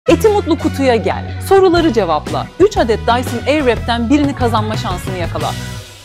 Eti Mutlu Kutu - Dyson Airwrap ile Mutluluğu Yakala!
Eti Mutlu Kutu'yu indir, çekilişe katıl, 3 adet Dyson Airwrap’ten birini kazanma şansını yakala! 🤩